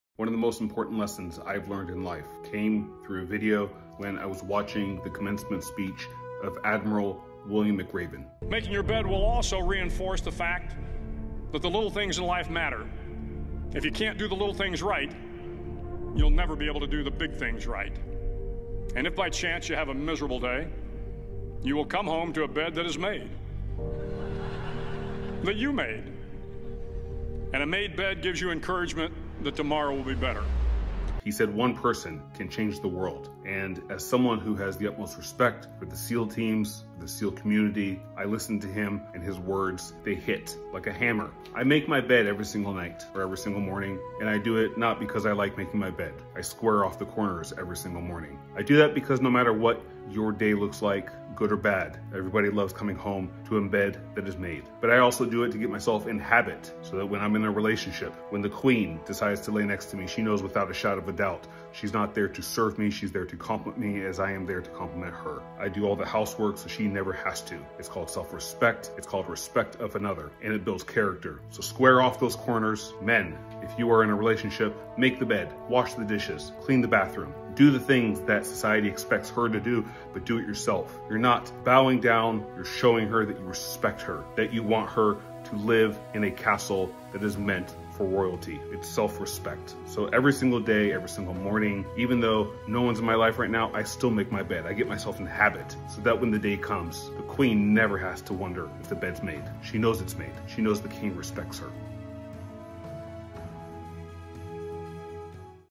Daily Bed-Making Habit - Navy SEAL's Secret To Success
Biggest life lesson from a Navy Seal, make the bed. Here is why. Making your bed every single day develops character, it's an act of self love, and if you're in a relationship with someone, it is a visual demonstration of respect for that person.

Everybody wants to come home to a bed that is made. So do yourself a favor, love yourself enough to come home to a bed that is made. It will change your day and it will change your life.